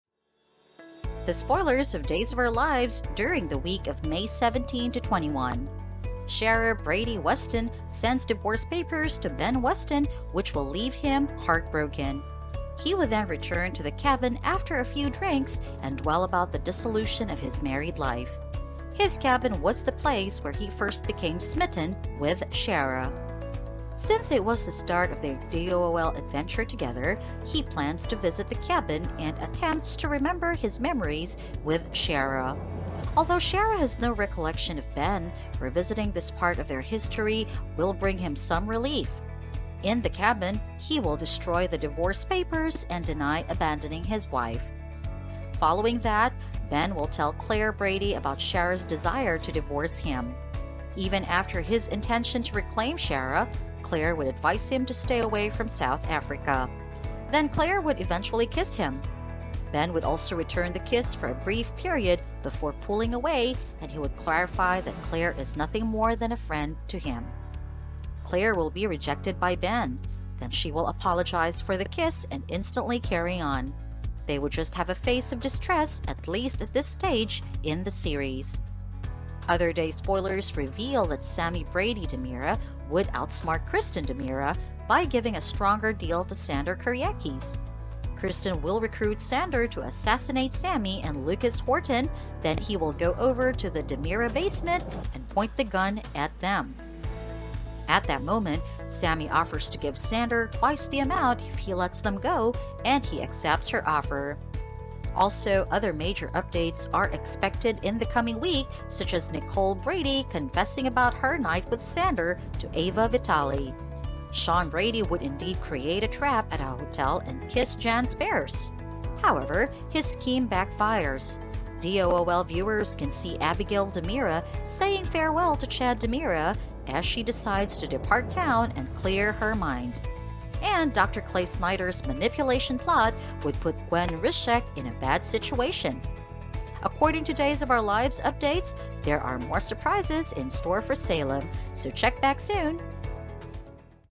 Days Of Our Lives Spoilers: Ben Fumes Ciara Divorce Proceedings, Sami Plays Kristen
Days of Our Lives spoilers for the week of May 17-21 reveal that Ben Weston burns the divorce papers sent by his wife, Ciara Brady Weston in his cabin. Claire Brady attempts to kiss Ben but gets rejected. Later, Sami Brady outsmarts Kristen DiMera by offering a higher price to Xander Kiriakis.

Thanks for coming by our channel!
We deliver Days of Our Lives Cast Updates & Spoilers here!